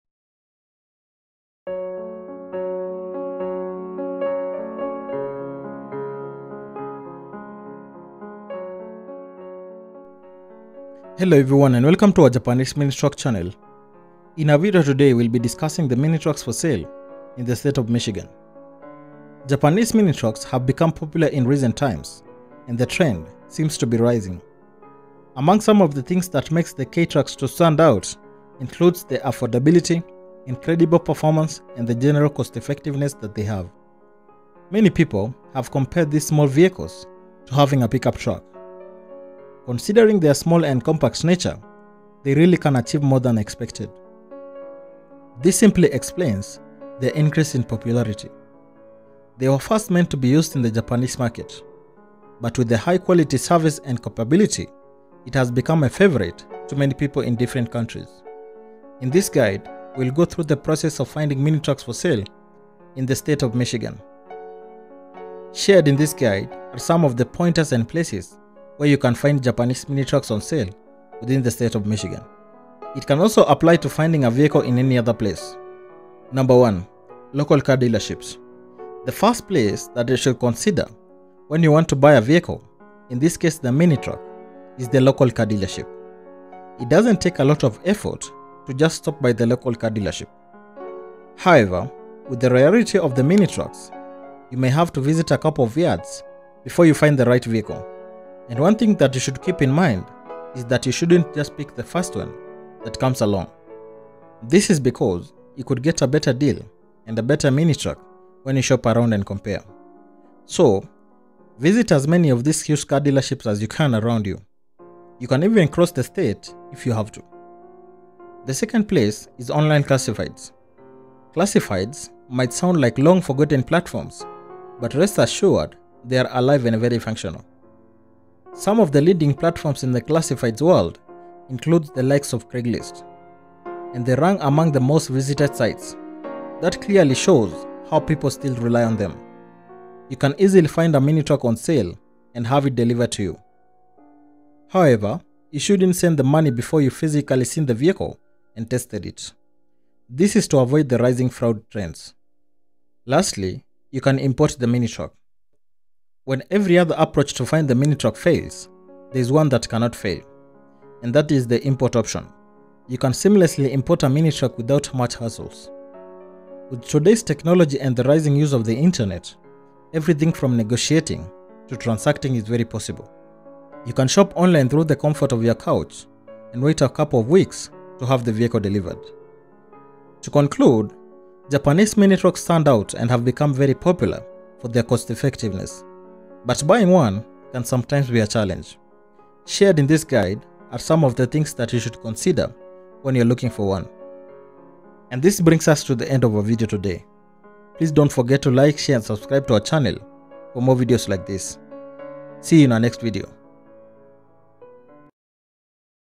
Mini Trucks For Sale in Michigan
Love Japanese mini trucks? In this guide, we will go through the process of finding mini trucks for sale in Michigan.